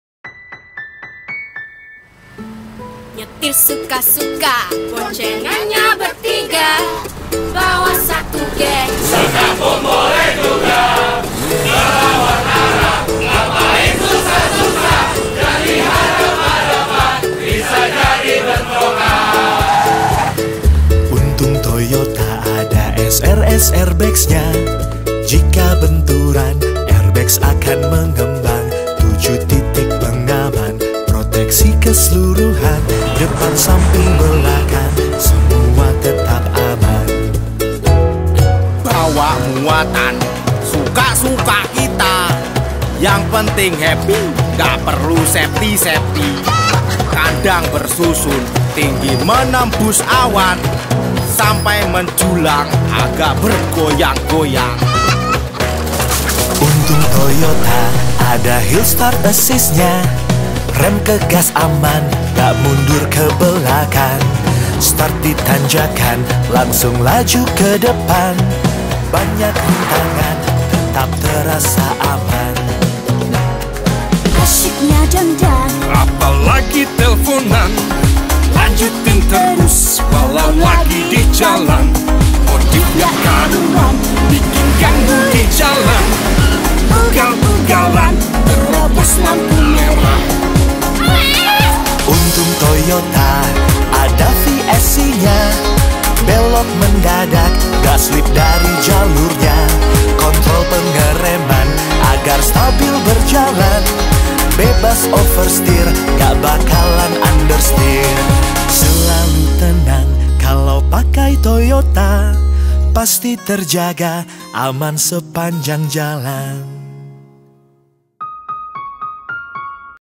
Be Safety Leaders : Amazing Road [FITUR KEAMANAN dan KESELAMATAN MOBIL TOYOTA]
It's time for you to be a safety leader, Friends!

Jadilah safety leader dengan kendaraan yang safety, agar bisa menjadi contoh bagi pengendara lain yang hobi berkendara suka-suka dan melupakan yang safety-safety.

*Konten mengandung adegan berbahaya dan tidak untuk ditiru